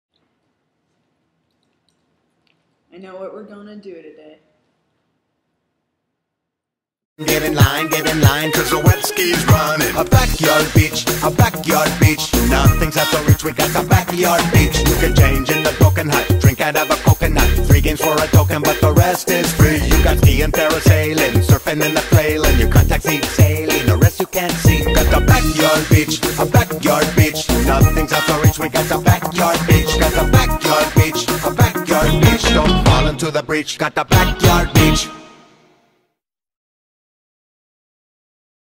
Backyard Beach
Backyard Beach with Kelly, Danny, Emily, and Jamie having fun!